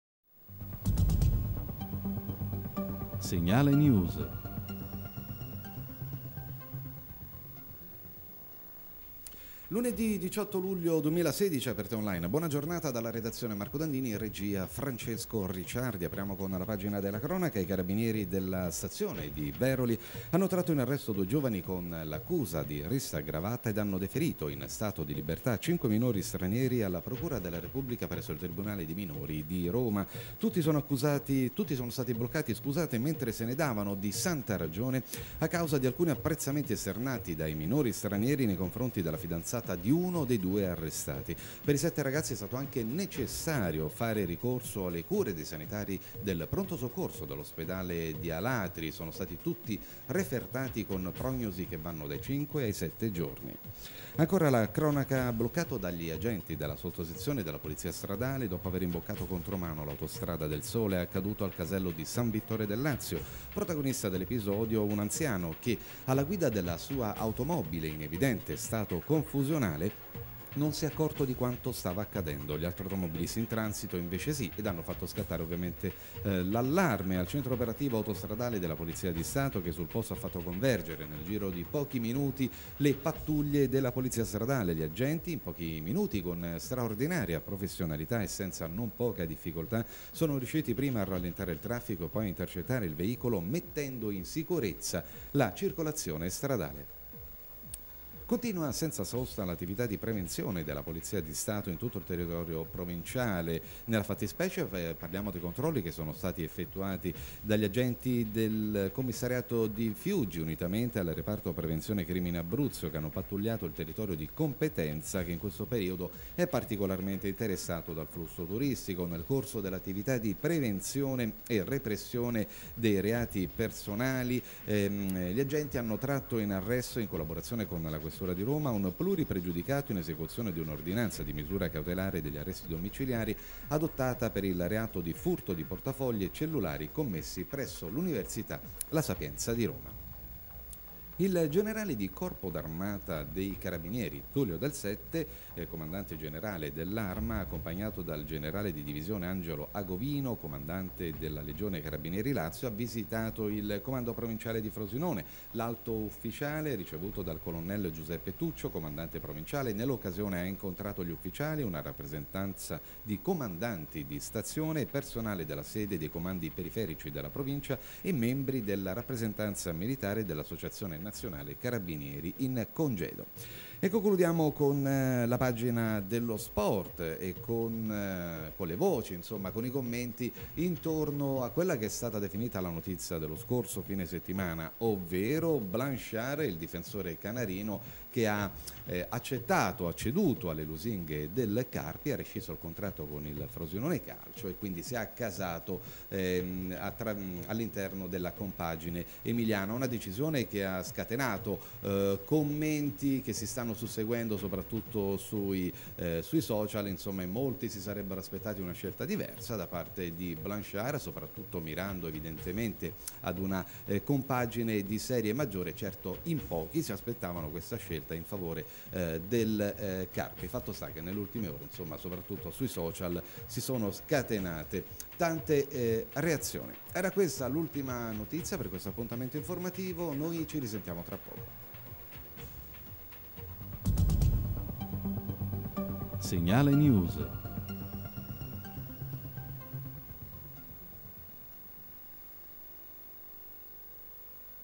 Segnale News Ore 9 18 luglio
Frosinone - Il Comandante Generale dell'Arma dei Carabinieri a Frosinone   Fiuggi - Controlli della polizia, un arresto   Anagni - Rapina in un bar dell'Osteria della Fontana  Cassino - Caso Acea: l'intervento del sindaco D'Alessandro   Pozzilli - Comi: "I Partiti non funzionano più"   Regione - Riforma Welfare, primi passi verso il reddito minimo garantito  Calcio - Blanchard sceglie il Carpi